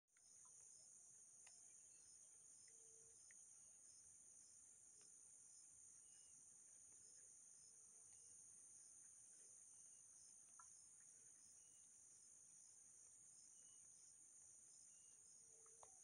Captura TEL. Atardecer 20/marzo/2023
De una de las mejores capturas que ha hecho mi teléfono fue este atardecer ocurrido un Lunes en un huerto donde estaba trabajando